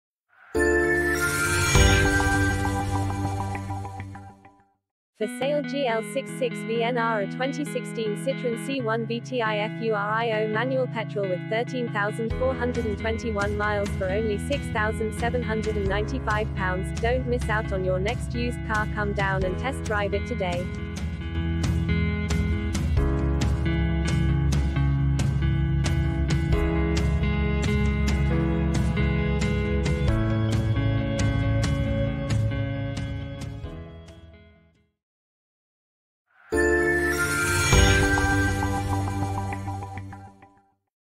For sale 2016 CITROEN C1 VTI FURIO MANUAL PETROL
For sale GL66VNR a 2016 CITROEN C1 VTI FURIO MANUAL PETROL with 13421 miles for only £6795. Don't miss out on your next used car come down and test drive it today!